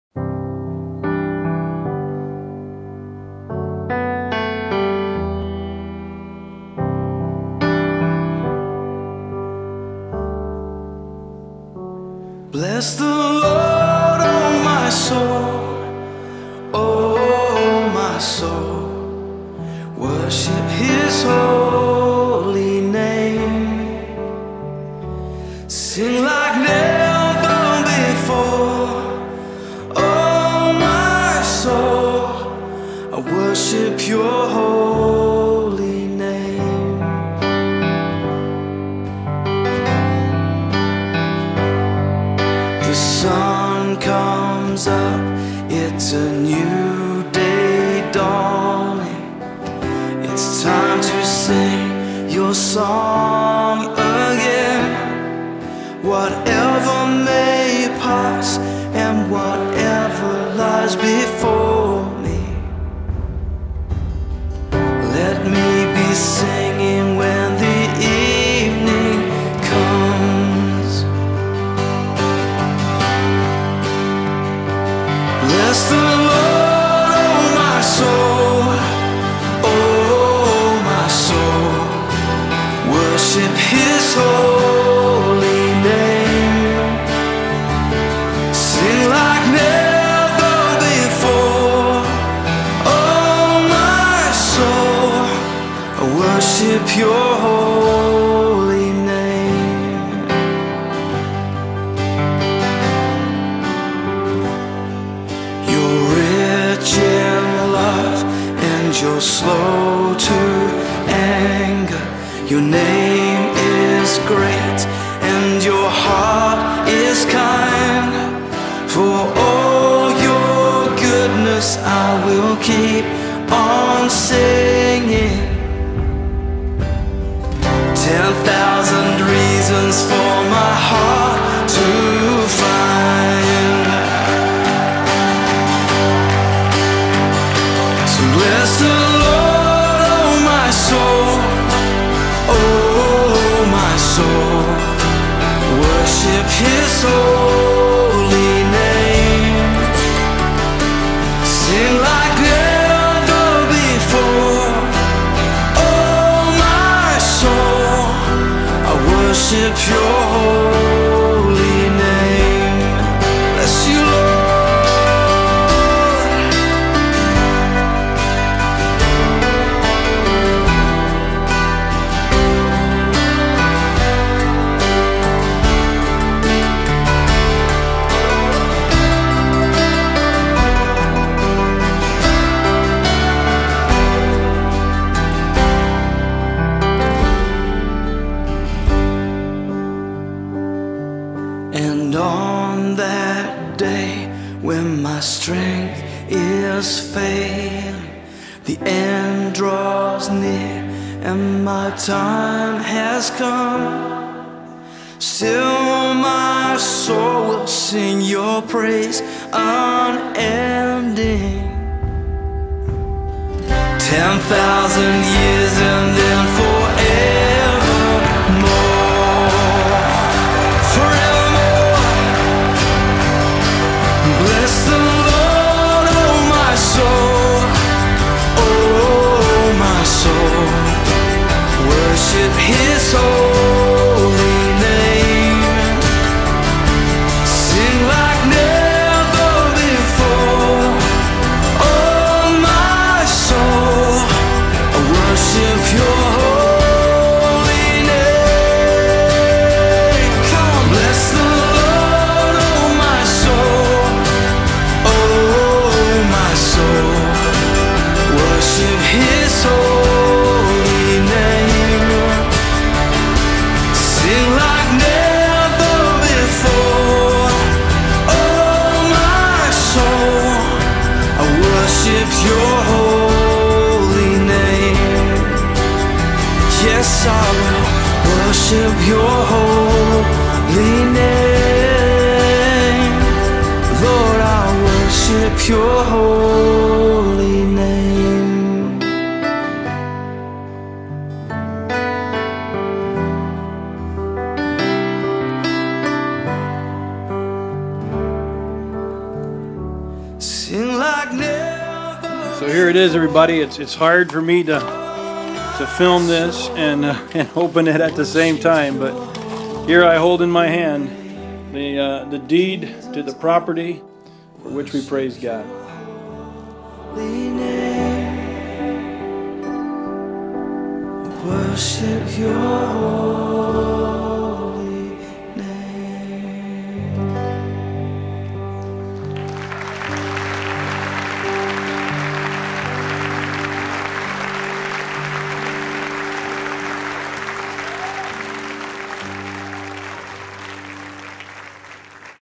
God Has Blessed Us This Past Year!
I put this slide show together to chronicle the construction of our newest building but as I worked on it I realized so many different ways that God had blessed us.  He provided so much for us so I try to show it in this video.